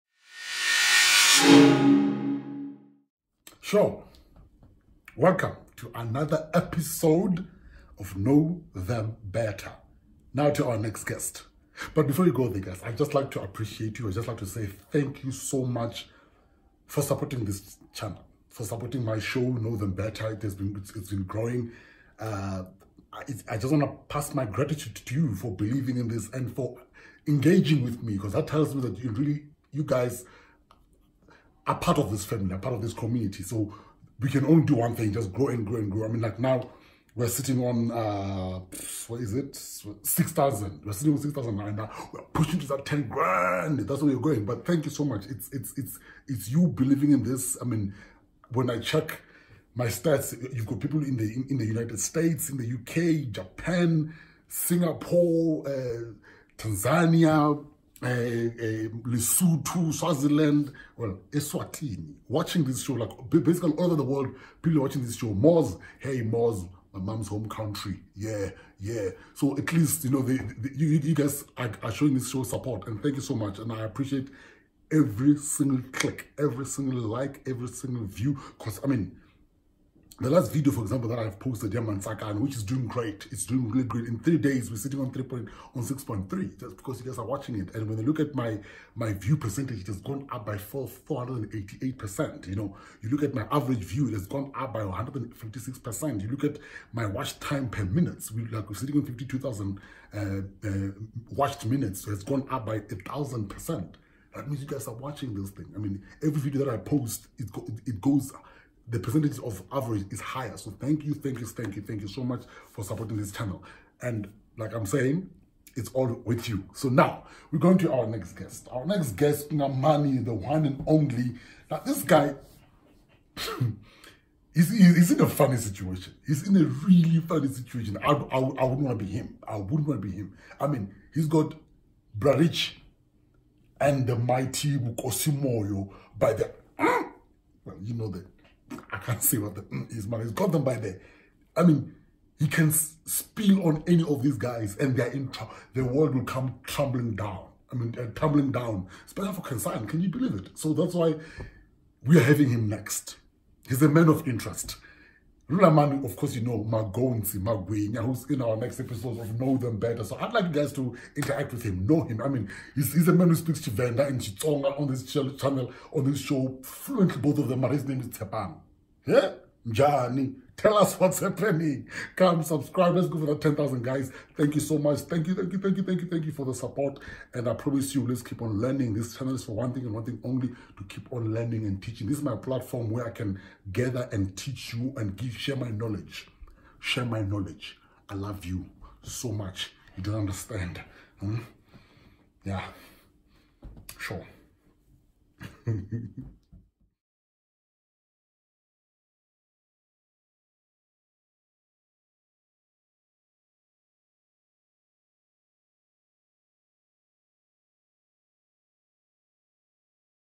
KnowThemBetterS1 Tshepang Selapyana - OUR NEXT GUEST: Episodes 12
Know Them Better Season 1

Ask our Stars anything you would like to know about them. The First 15 question on the comments section will ask our starts. In this Episode, we calling for your question to ask Musai anything you like to ask him.